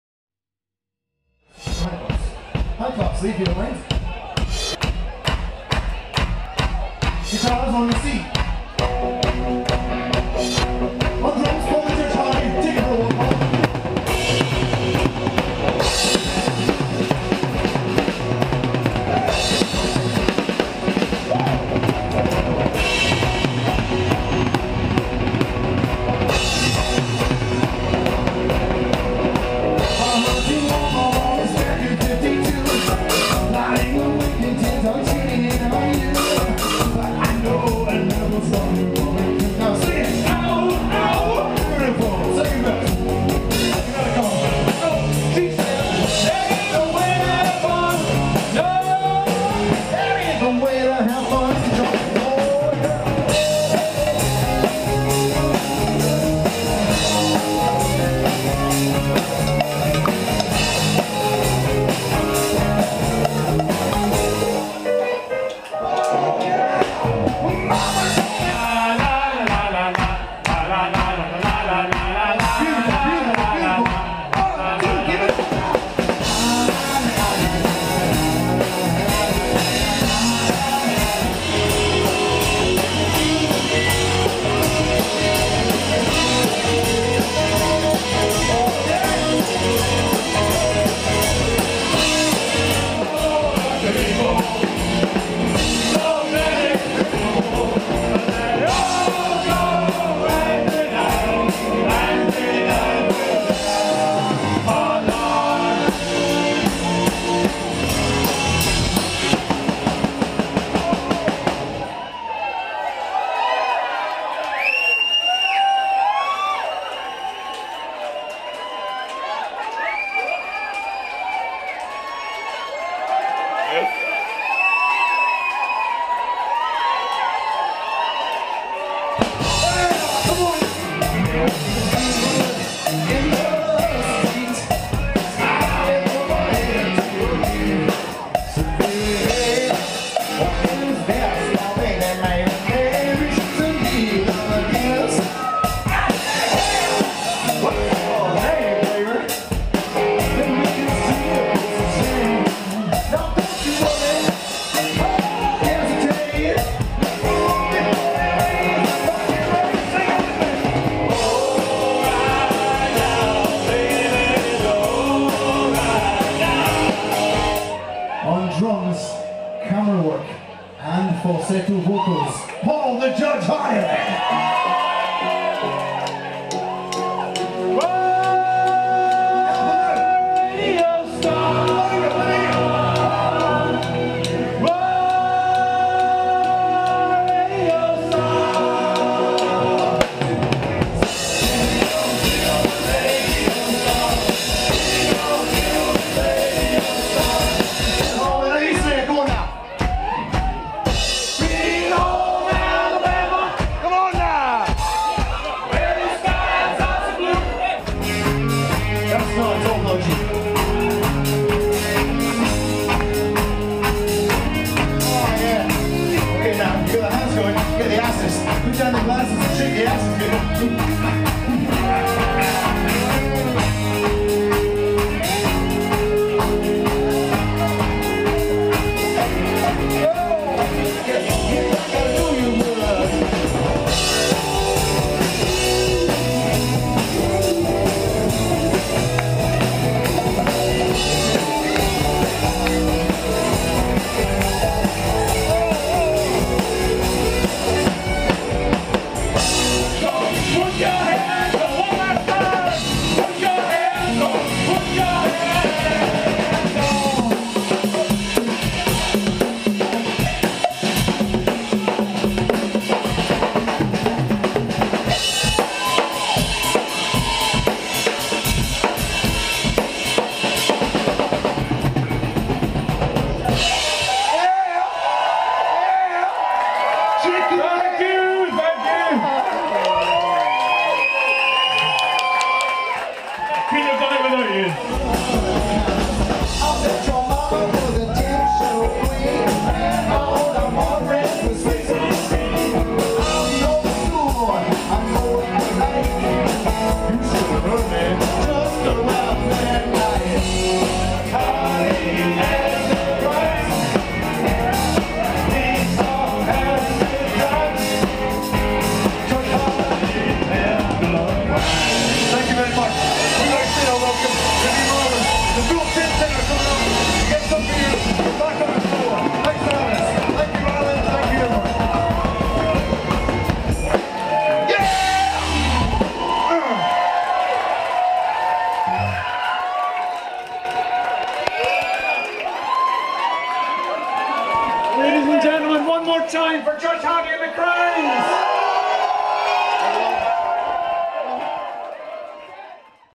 Judge Hardie & The Crimes : Oktoberfest 2016
Judge Hardie & The Crimes are back!

Dundee Rotary Oktoberfest, Saturday 24th Sept, 2016